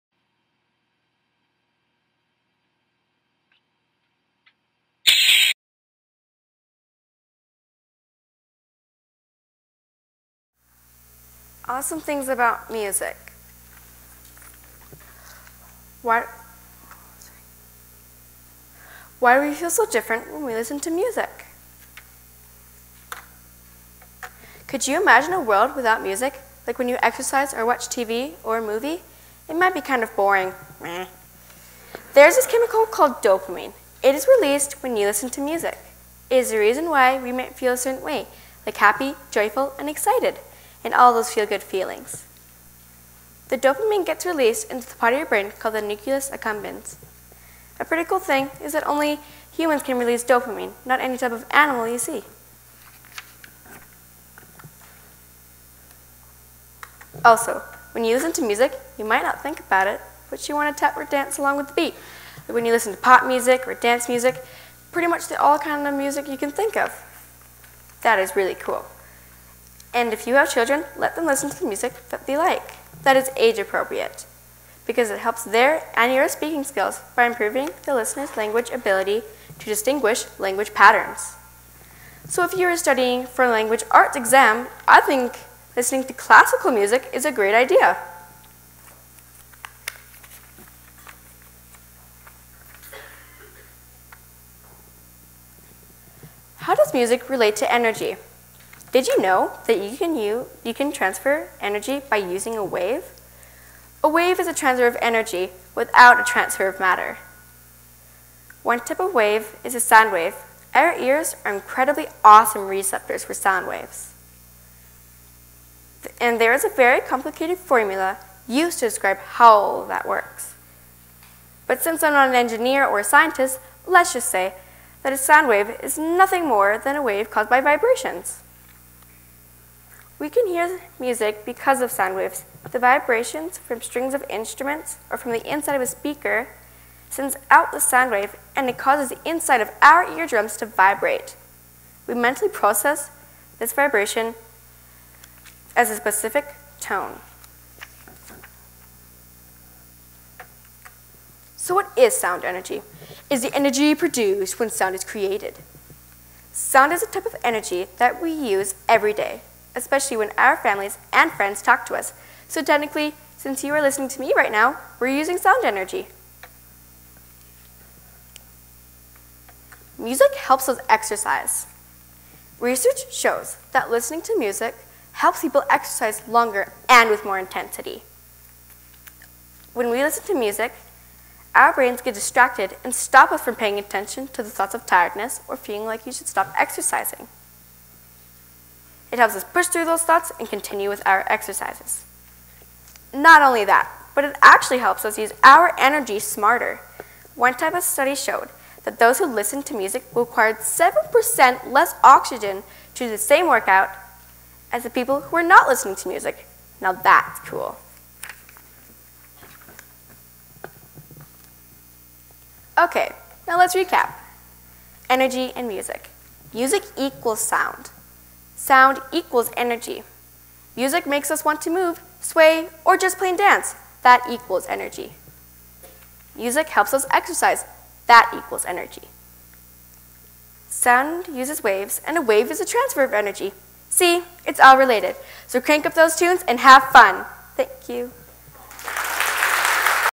Awesome Things about Music | Braelyn Roy | TEDxRundleAcademy
Did you know music is a type of energy? Braelyn Roy, in a short and energizing talk, explores the power that music has over our frame of mind.

Braelyn Roy is a grade 6 student who investigates how music is  a powerful force in our lives. She is a student athlete, ambassador and enthusiastic member of her school's TED-Ed Club.